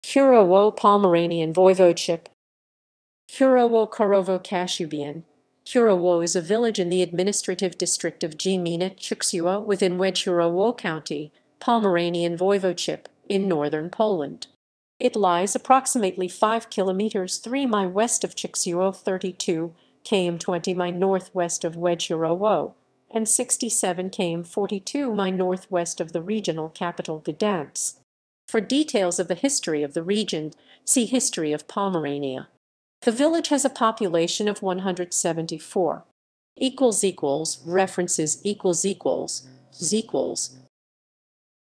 Kurowo, Pomeranian Voivodeship
Kurowo [kuˈrɔvɔ] (Kashubian: Kurowò) is a village in the administrative district of Gmina Choczewo, within Wejherowo County, Pomeranian Voivodeship, in northern Poland. It lies approximately 5 kilometres (3 mi) west of Choczewo, 32 km (20 mi) north-west of Wejherowo, and 67 km (42 mi) north-west of the regional capital Gdańsk.
For details of the history of the region, see History of Pomerania.
The village has a population of 174.

== References ==